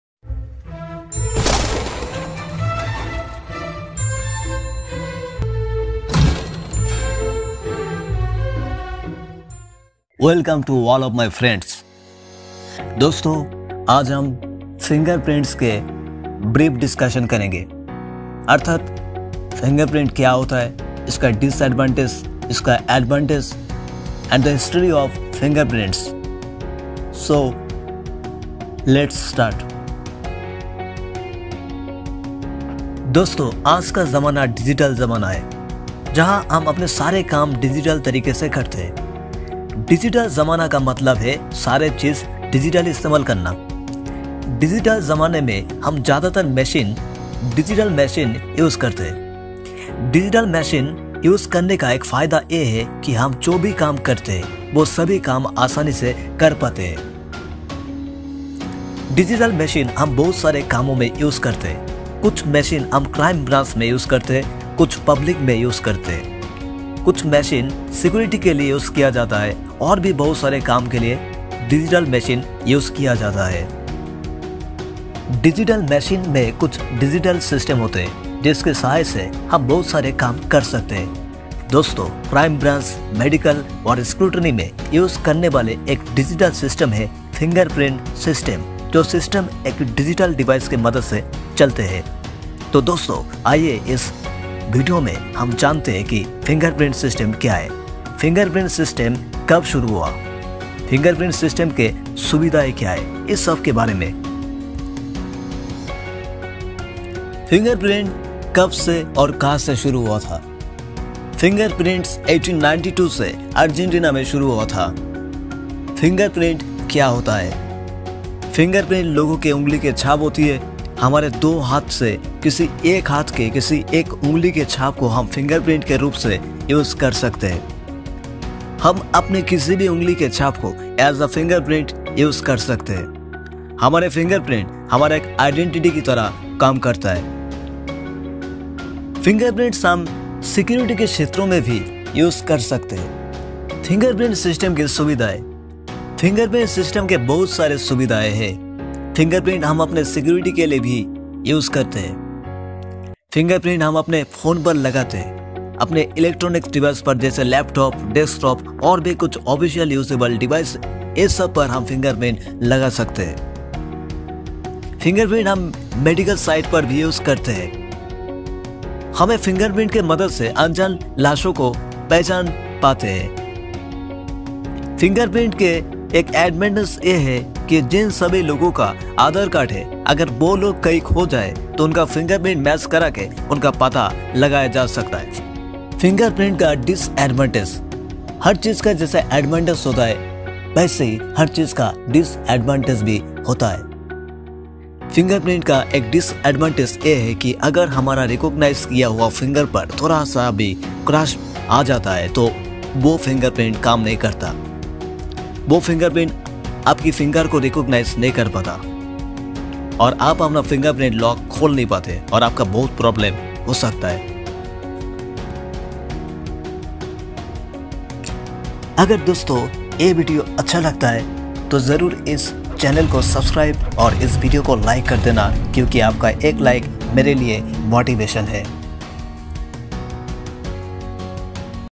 What is Fingerprint System?.फिंगरप्रिन्ट सिस्टेम क्या होता है?फिंगरप्रिन्ट सिस्टेम के फायदे।
History of Fingerprint System.
Advantage of fingerprint System.
Disadvantage of Fingerprint System.
Uses of fingerprint in TV,Laptop,Medical,Crime Scene etc.